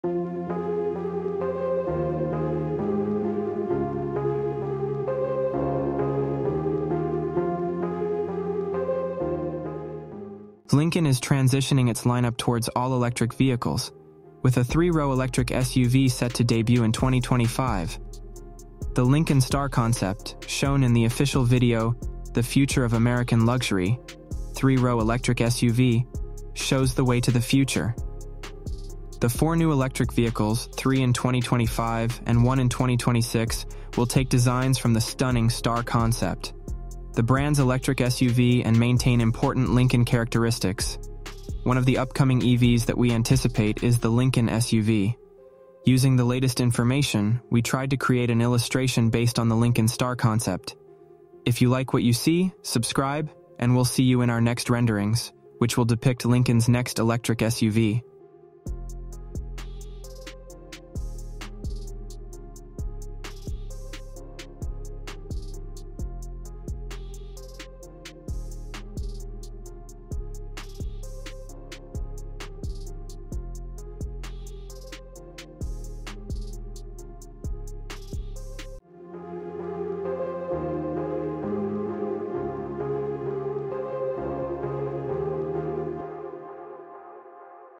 The 2025 Lincoln First Three-Row Electric SUV - Luxury & High-Tech
A 2025 three-row electric SUV will join Lincoln's all-electric portfolio.the Lincoln Star Concept reveals the future. Four new electric vehicles (three in 2025 and one in 2026) will draw from the beautiful Star concept. Lincoln's electric SUV retains key features. We expect the Lincoln SUV EV.we tried to illustrate the Lincoln Star Concept.